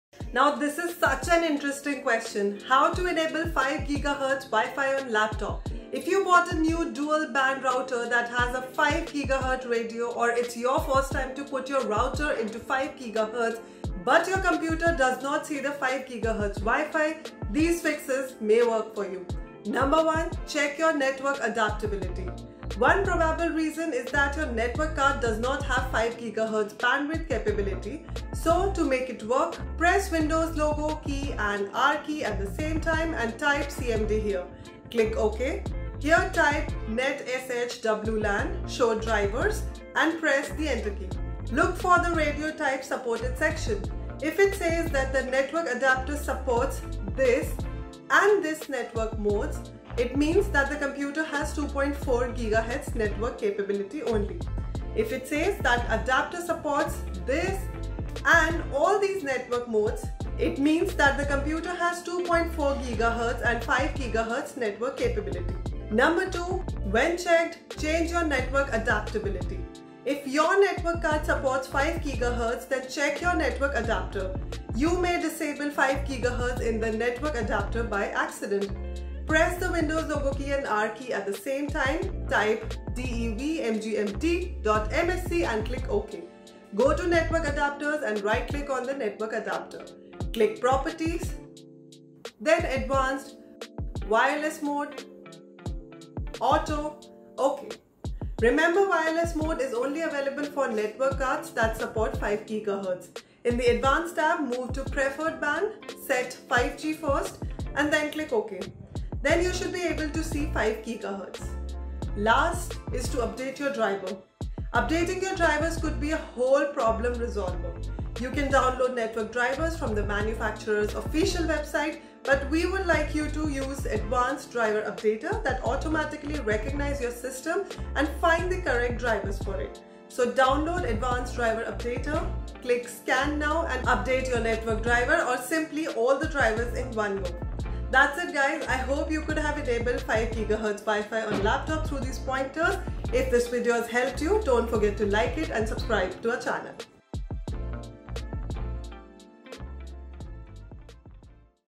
How To Enable 5GHz Wi Fi On Laptop | Change Wi-Fi Band From 2.4GHz to 5GHz
If you want to enable 5GHz Wi Fi on your laptop, you will need to have a compatible wireless adapter. For most laptops, this will be a built-in adapter. To check if your adapter supports 5GHz, you can go to the Device Manager. Look for the Network Adapters section and expand it. Find your adapter, right-click it, and select Properties. Go to the Advanced tab and look for the 802.11n Channel Width setting. If it is set to Auto, change it to 40MHz. If you don't see the setting, your adapter does not support 5GHz.